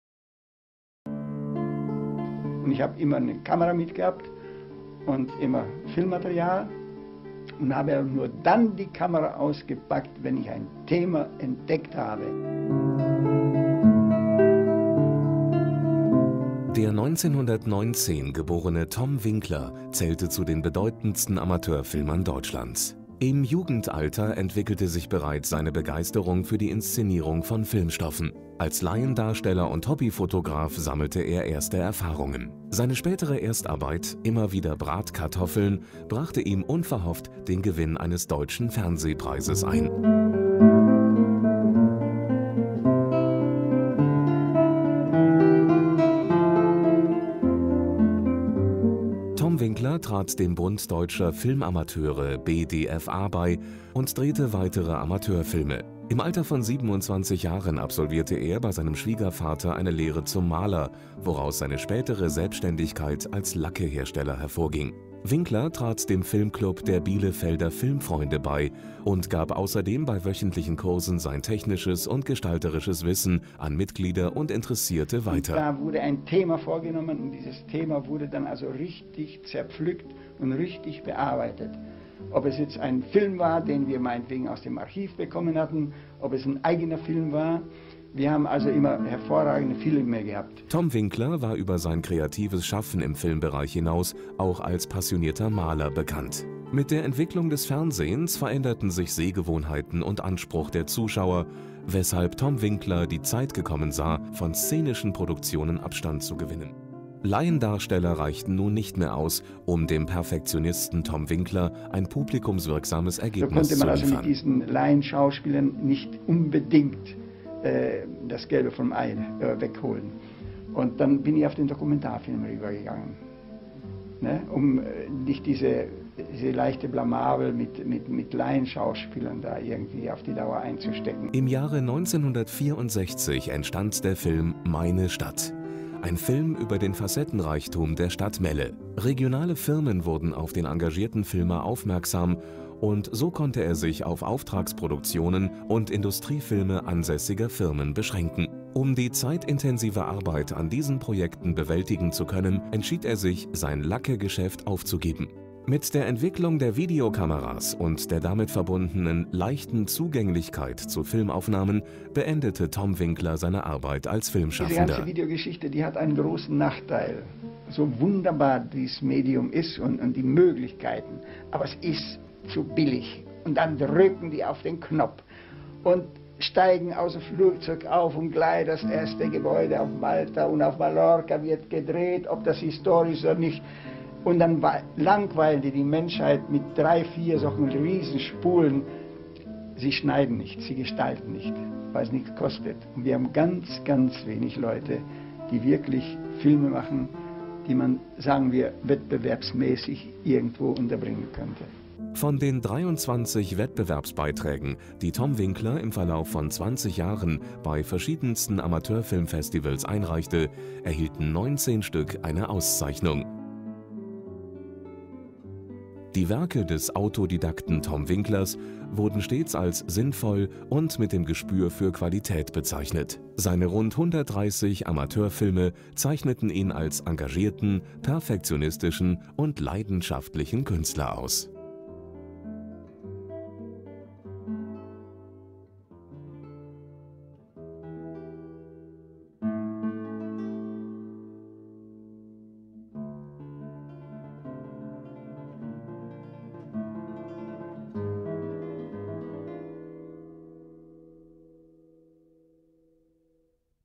Tom Winkler Maler Filmer Künstler
Filmportrait über den Filmemacher, Maler und Künstler Tom Winkler